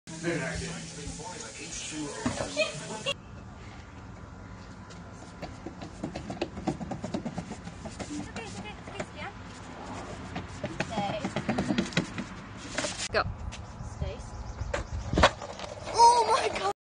Dog rides a skateboard!!!!!!!!!
Bentley the dog rides a skateboard